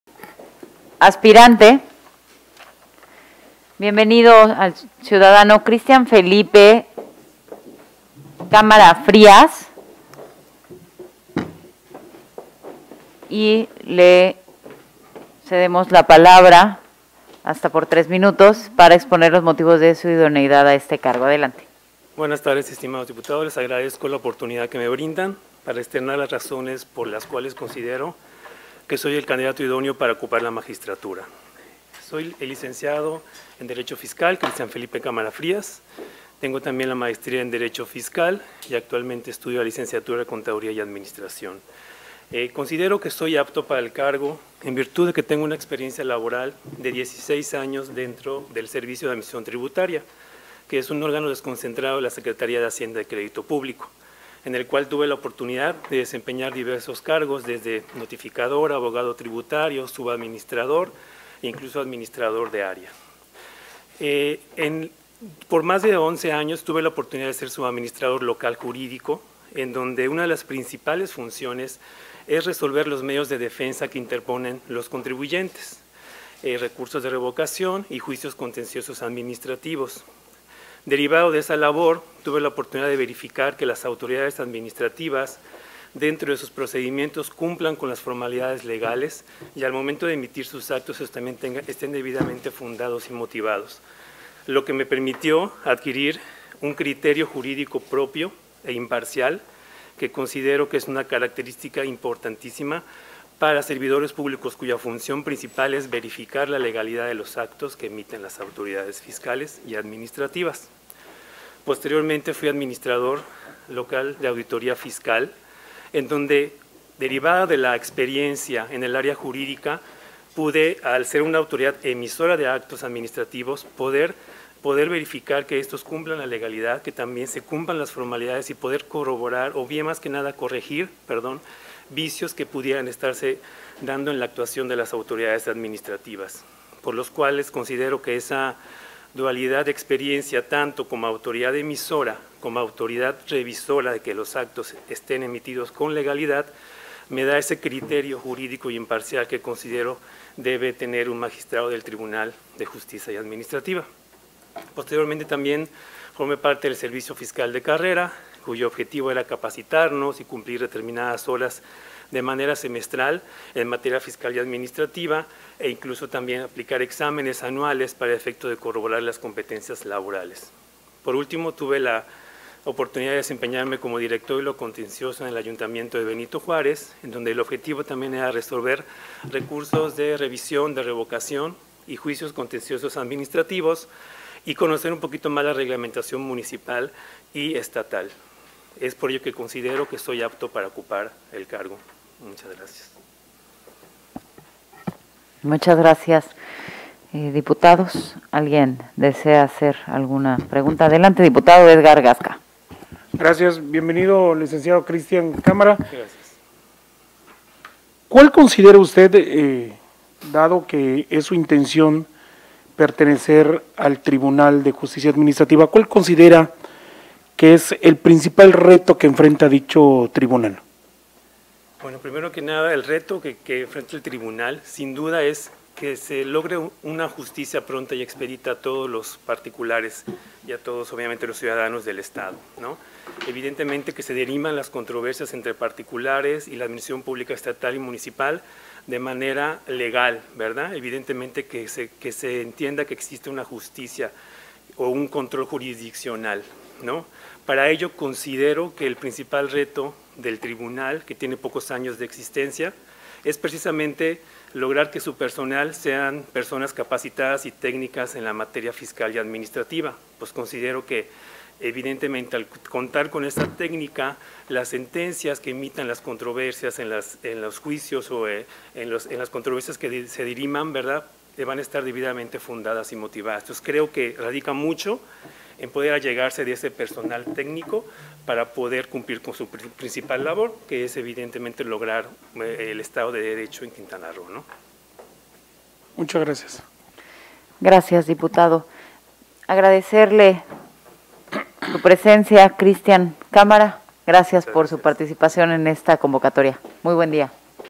Entrevista C. Christian Felipe Cámara Frías. Comisión Anticorrupción. (18/Ene/2022)
Entrevista al C. Christian Felipe Cámara Frías, aspirante para ocupar la titularidad de la Magistratura del Tribunal de Justicia Administrativa del Estado de Quintana Roo, ante la Comisión Anticorrupción, Participación Ciudadana y Órganos Autónomos de la XVI Legislatura.